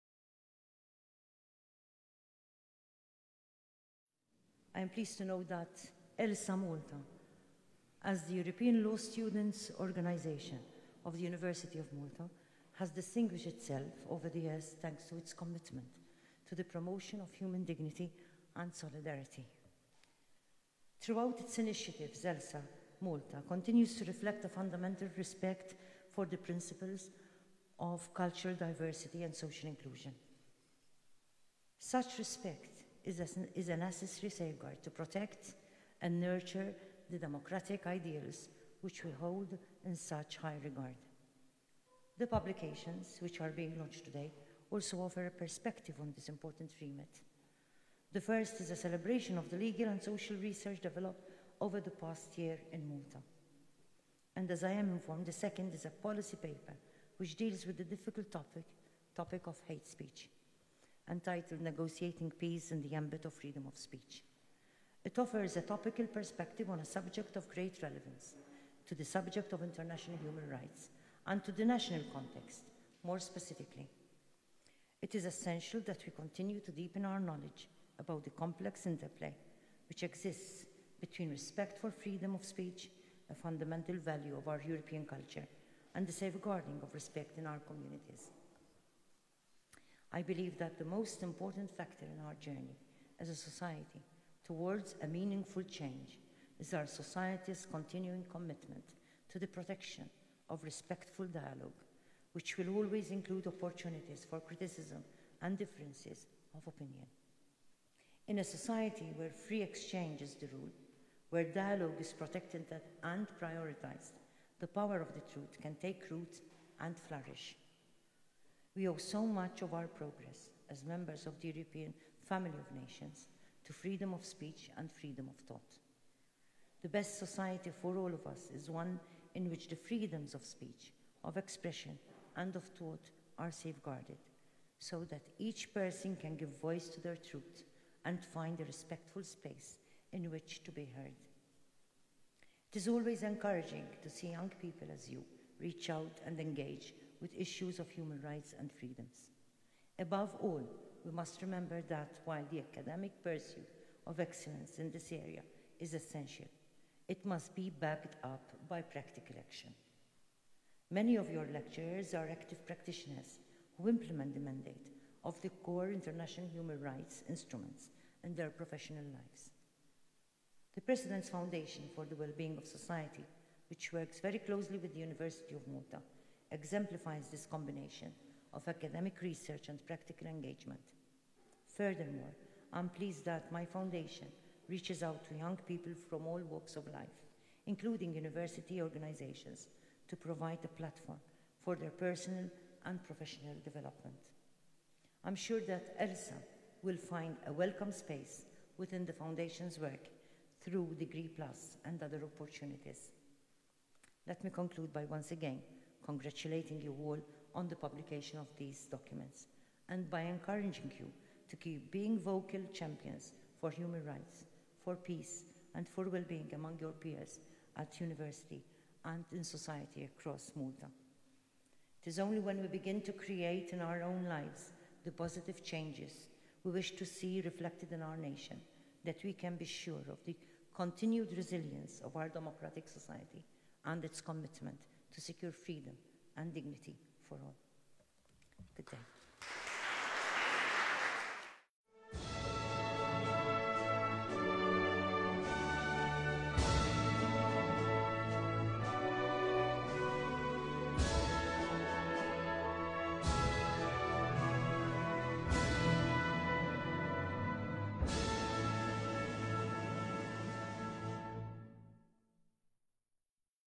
Speech during the launch of two publications by ELSA Malta
The Annual ELAA Malta Law Review and a policy paper on Hate Speech